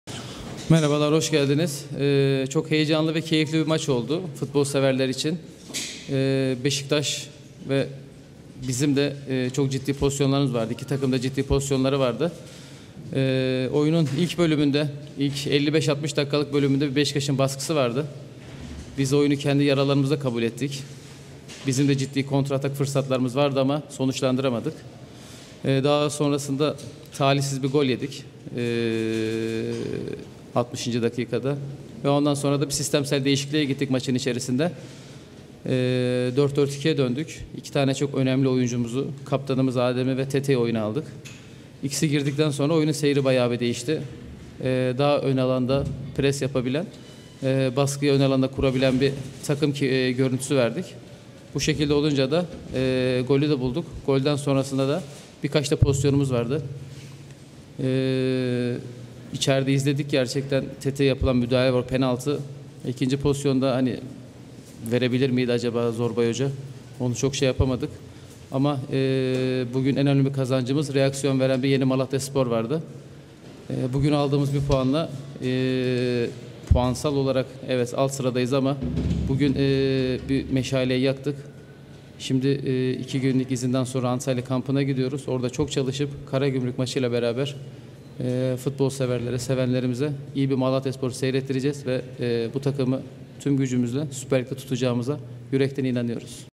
Şener Gençtürk: Çok heyecanlı ve keyifli bir maç oldu
Beşiktaş beraberliği sonrasında müsabakanın değerlendirmesini yapan Yeni Malatyaspor Yardımcı Antrenörü Şener Gençtürk, "Çok heyecanlı ve keyifli bir maç oldu” dedi.